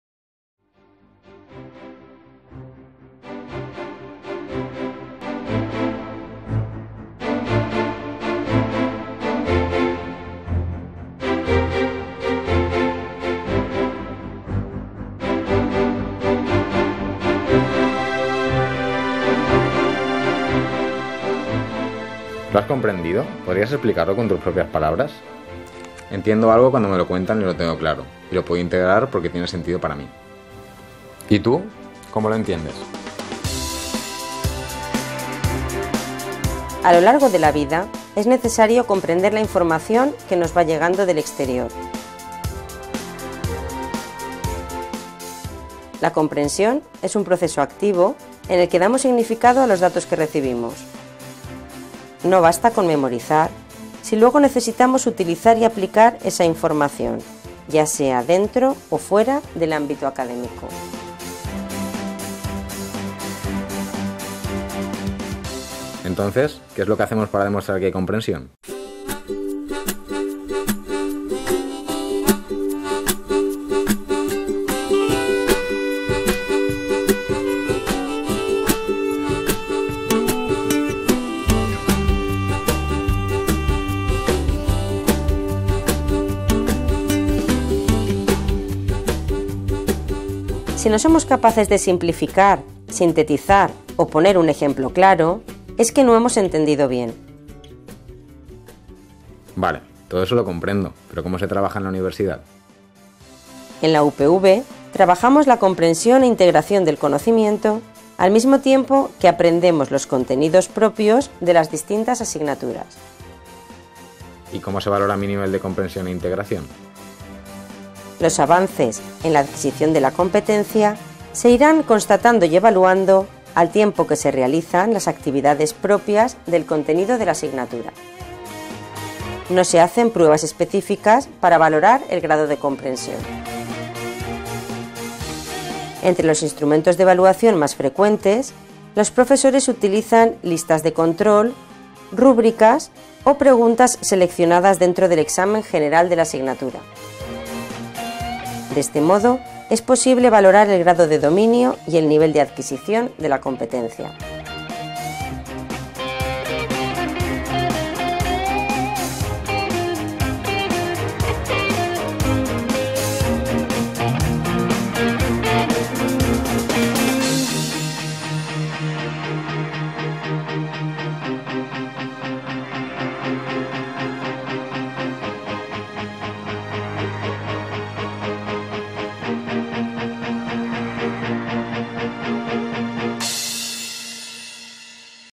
Competencias Transversales UPV: Comprensión e integración
Los procesos de comprensión e integración del conocimiento pueden resultar algo de carácter abstracto para un estudiante. Con este vídeo se pretende clarificar en qué consiste la competencia y cómo se puede desarrollar, así como el modo en que se trabaja y evalúa en la Universitat Politècnica de València.